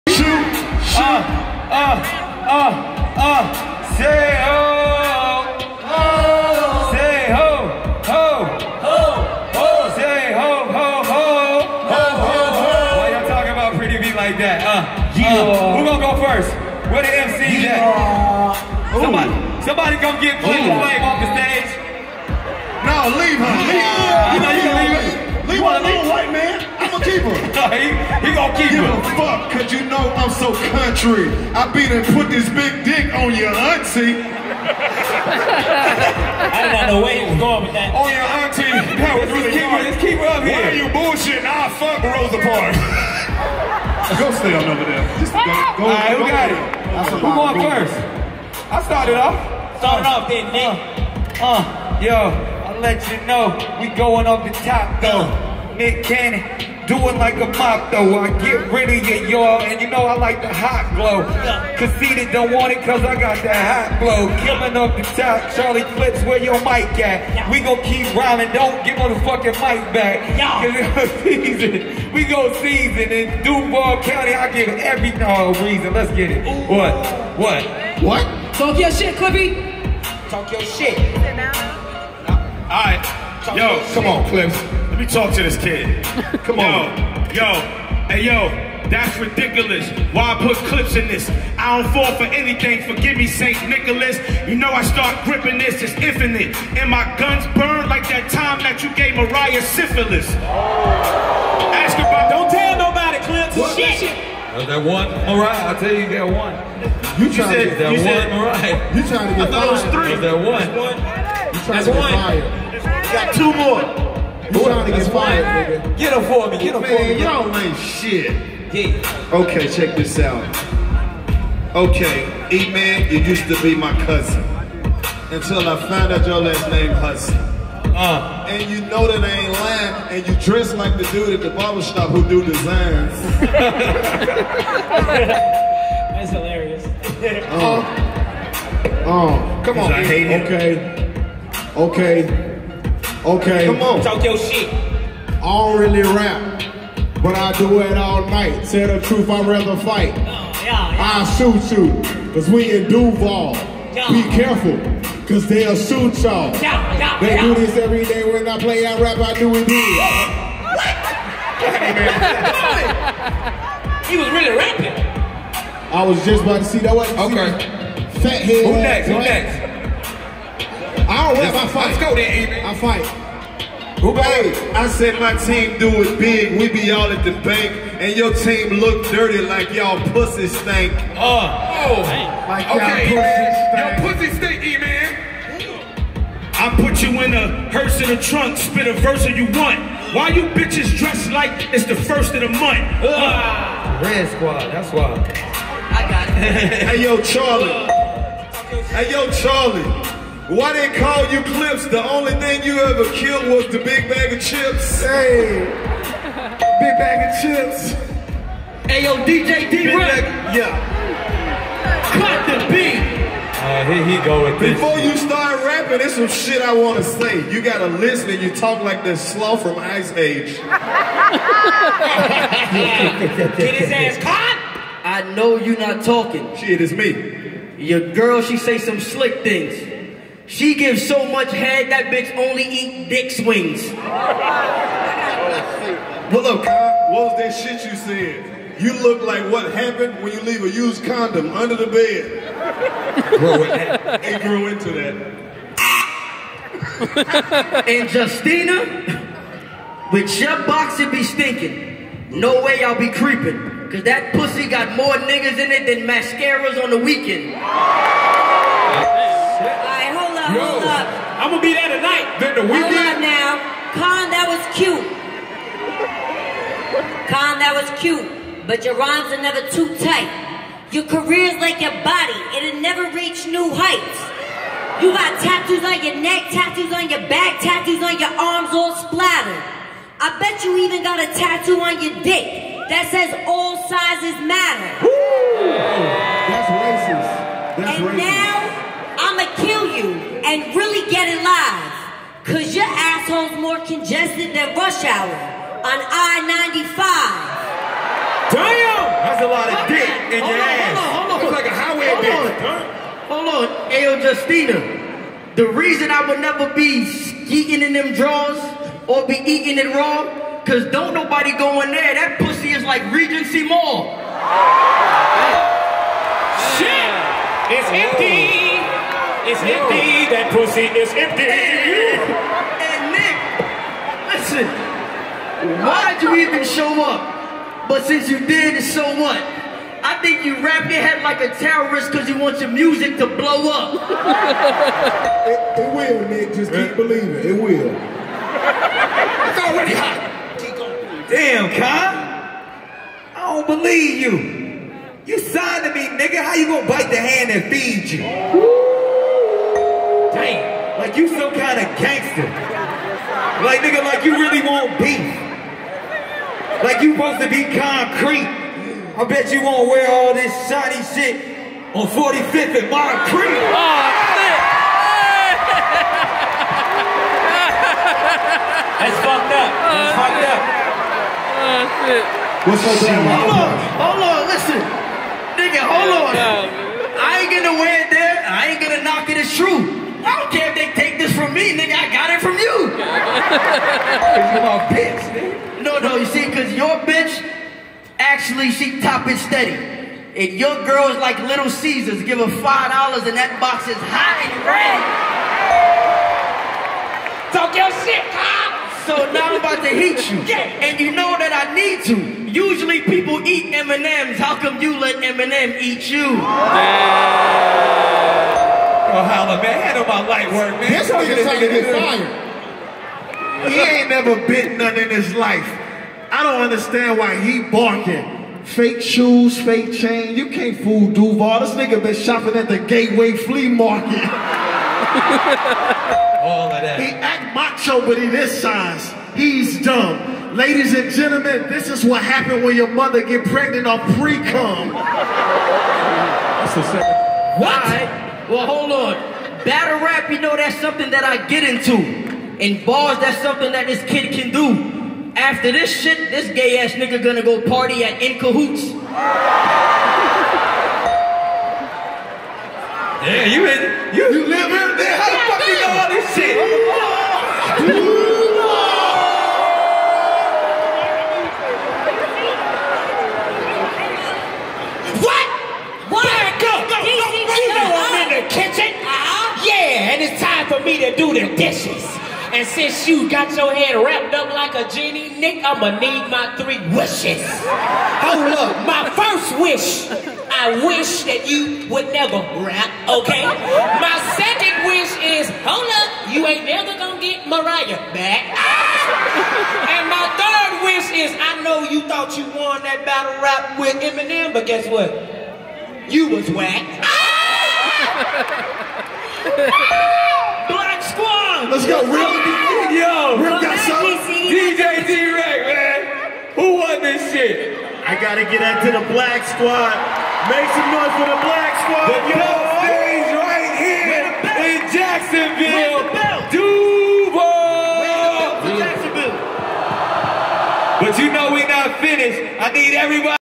Wild N Out Jacksonville Tour 2020 Wild Style
Wild style part of the show in Jacksonville, Fl. Vystar Arena.